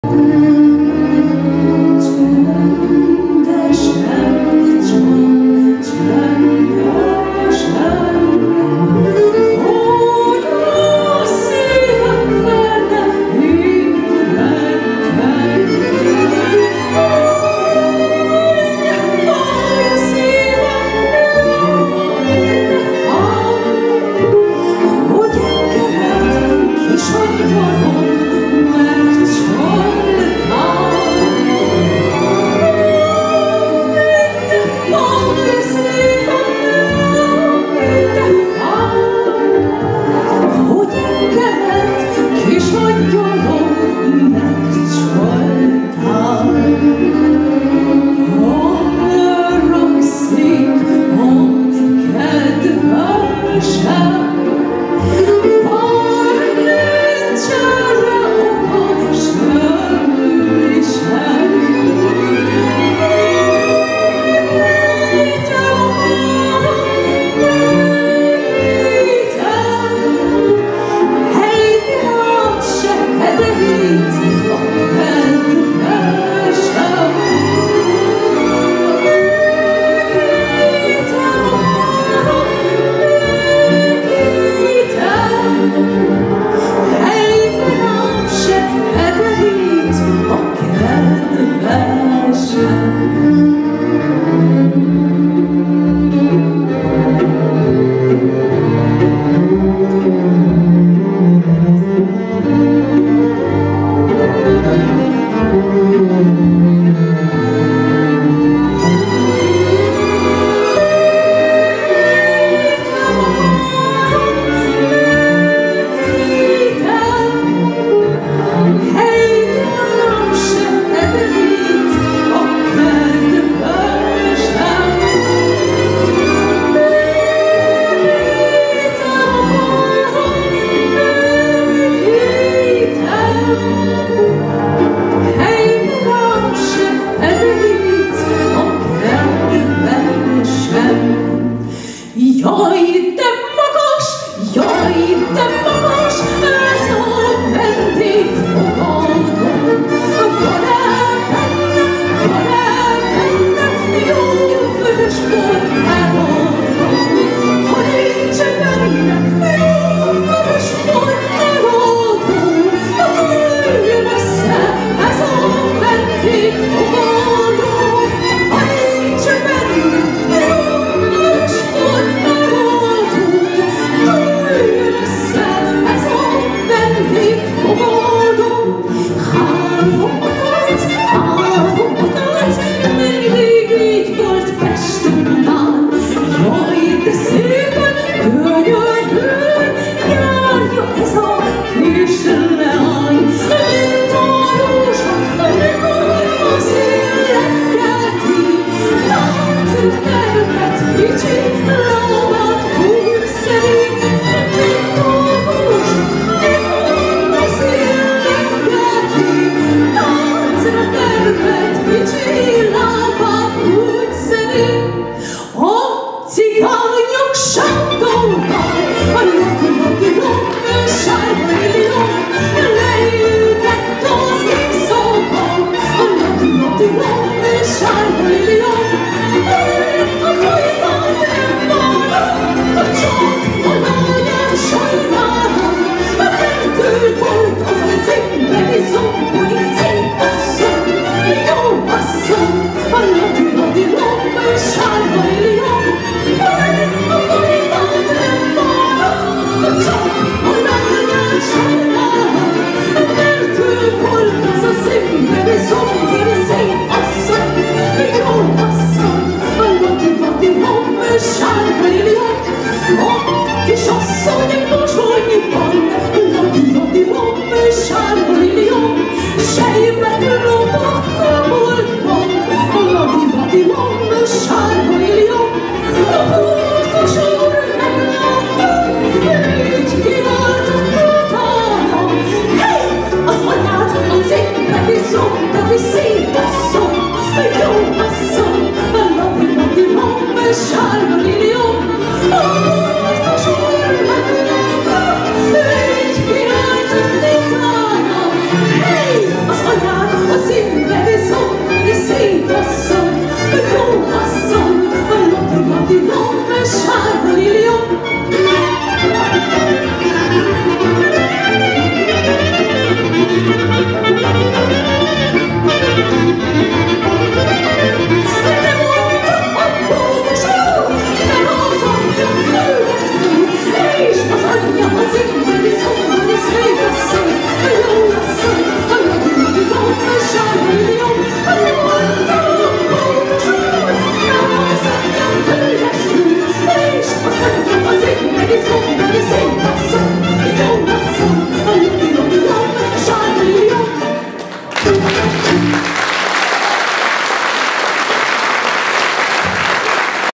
Concert Agnes Visser in Theater Diligentia (Fragment)
Részlet Visser Ágnes lemezbemutató koncertjéből. Hága, Theater DIligentia